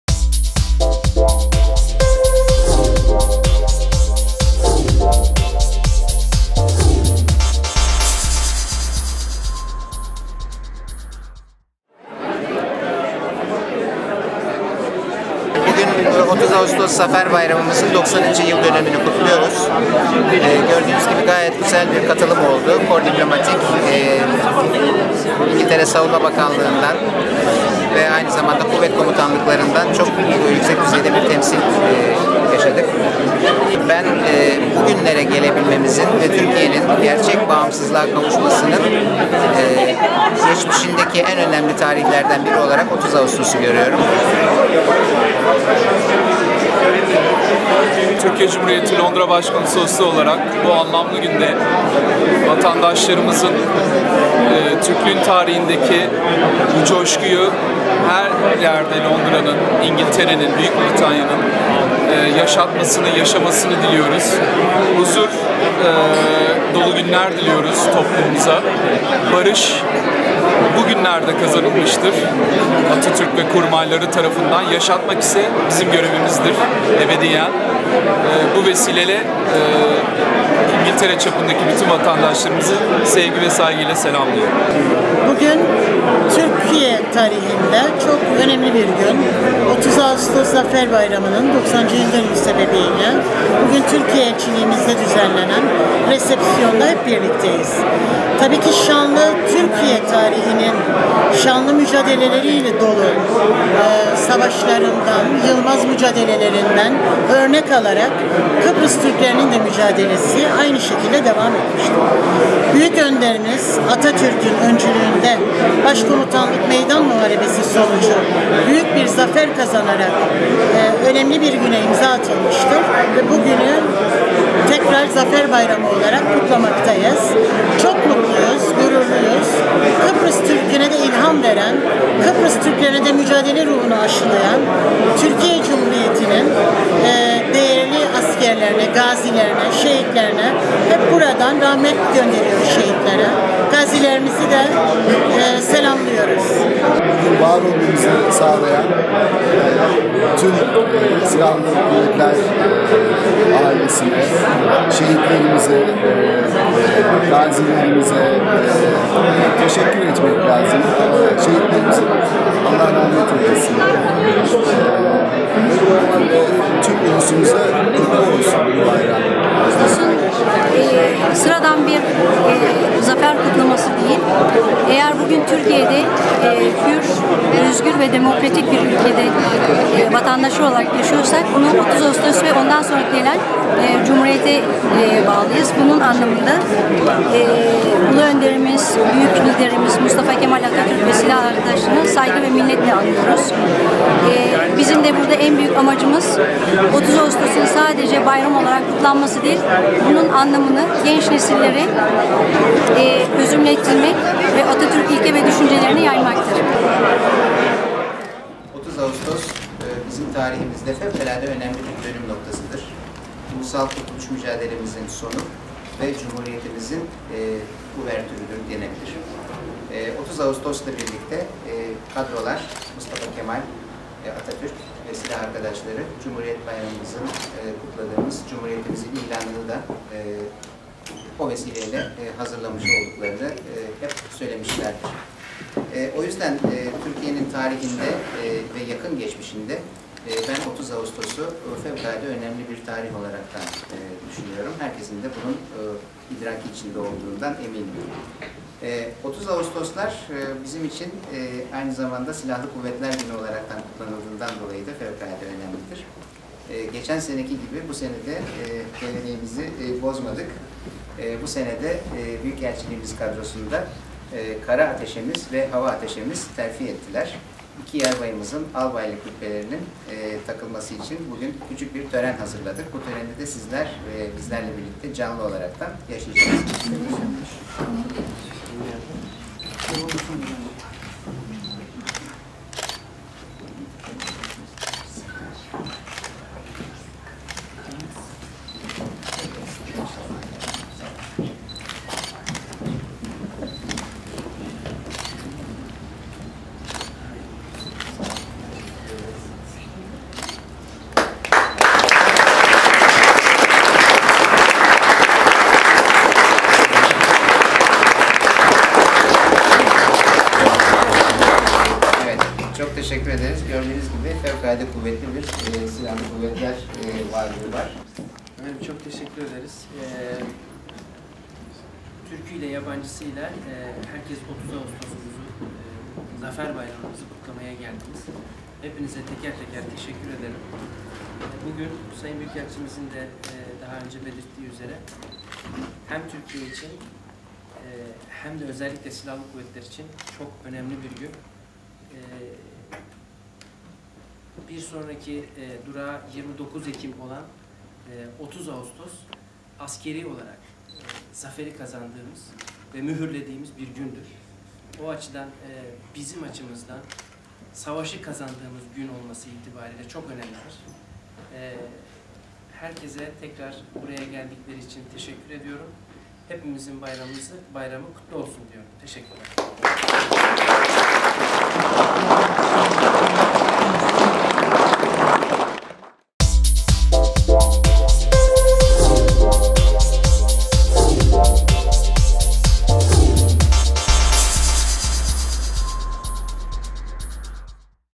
LTR | Londra Türk Radyosu | 30 AGUSTOS BÜYÜKELÇILIK RESEPSiYONU
LTR  |  Londra Türk Radyosu  | London Turkish Radio | 30 AGUSTOS BÜYÜKELÇiLiK RESEPSiYONU

30 Agustos Zafer Bayramimizin 90. yil dönümü ve Türk Silahli Kuvvetleri Günü,  Londra Büyükelçilik rezidansinda verilen resepsiyonla kutlandi.

T.C. Londra Büyükelçisi Sn. Ünal ÇEVIKÖZ ve Silahli Kuvvetler Atesesi Kurmay Albay Erdal ERGÜN'ün ev sahipliginde yapilan resepsiyona çok sayida Büyükelçilik ve Konsolosluk çalisani katildi. Londra Türk Radyosu yayin ekibinden Müge Bakircioglu ve Deniz  Erdogan(Djmoom), Sn. Büyükelçi Ünal ÇEVIKÖZ'le 30 Agustos Zafer Bayrami hakkinda kisa bir söylesi yapti.

Ingiltere Savunma Bakanligindan ve ayni zamanda Kuvvet Komutanliklarindan yüksek düzeyde bir temsil gerçeklestigini belirten Sn. Büyükelçi ÇEVIKÖZ yaptigimiz söyleside " Zafer Bayramimizin 90. yildönümünü kutluyor olmamiz bence fevkalade önemli, 30 Agustos' un bizim tarihimizde önemli bir dönüm noktasi oldugu herkes tarafindan biliyor. Cumhuriyetin hazirlanmasi ve ilani için Ulusal Kurtulus Mücadelemizin sona ermesi beklenmis, 30 Agustos Zaferi ile de hazirlanmisti, bunun için Zafer Bayrami' ni büyük bir cosku ile kutluyoruz. Ben, bugünlere gelebilmemizin ve Türkiye' nin gerçek bagimsizligina kavusmasinin geçmisindeki en önemli tarihlerinden biri olarak 30 Agustos' u görüyorum ve yüksek düzeyde katilim olmasindan çok büyük memnuniyet duyuyorum. " seklinde konusmasina devam etti.

Sayin Büyükelçi Çeviköz' ün konusmasini takiben Silahli Kuvvetler Atesesi Erdal ERGÜN ile birlikte Kara Askeri Atesesi Topçu Kurmay Albay Erdinç Kocayanak ile Hava Atesesi Hava Pilot Kurmay Albay Ömer ÖZKAN' a Albay rütbeleri takildi.